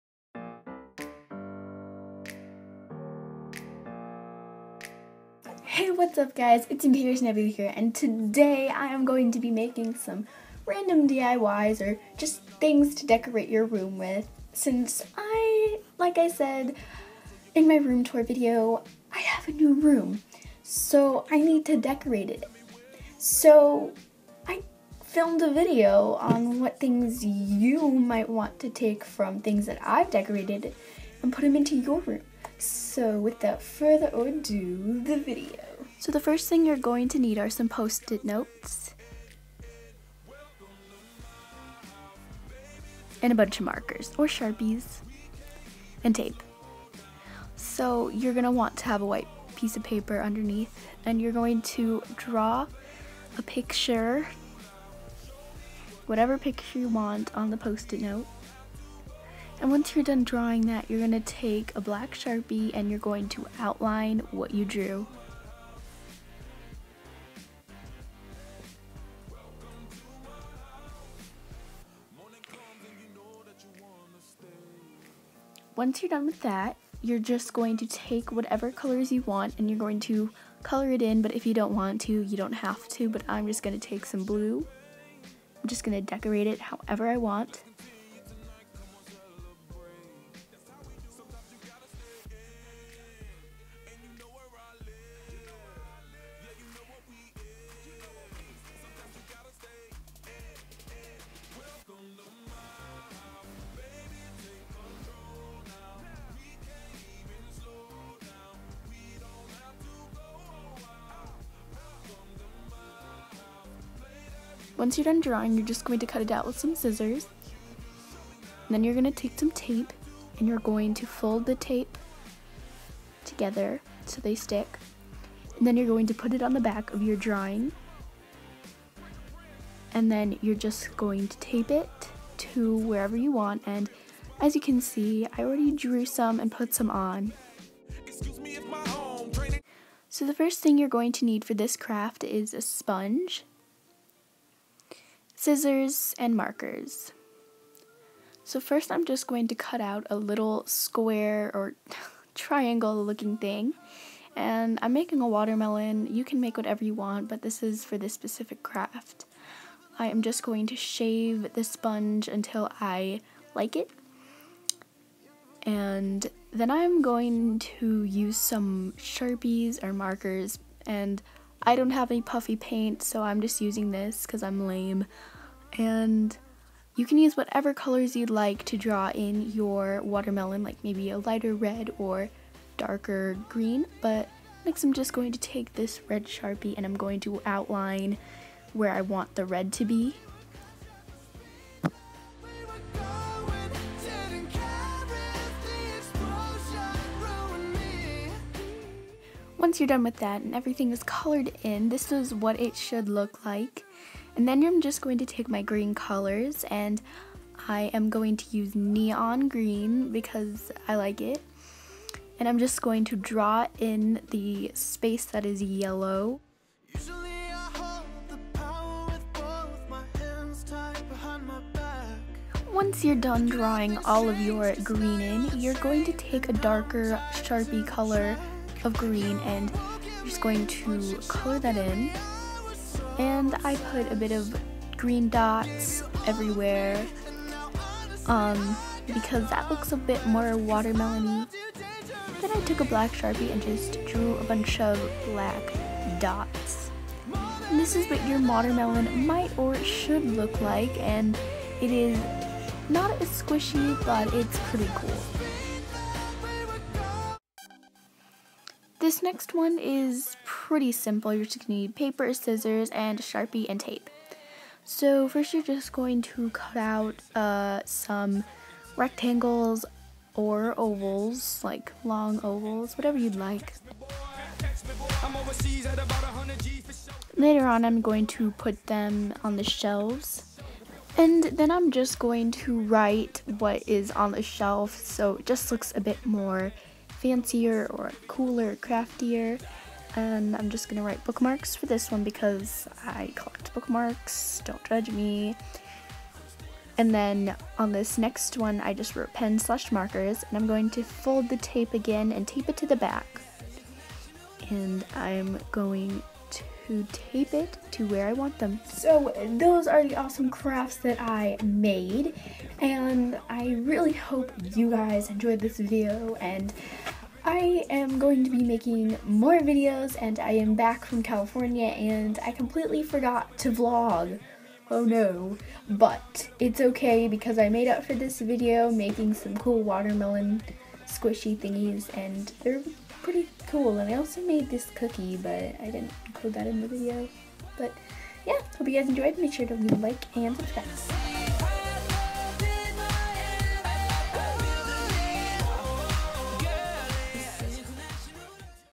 DIY Room Decor!
So today I make some pretty cool crafts that you can also make! Hope you enjoyed!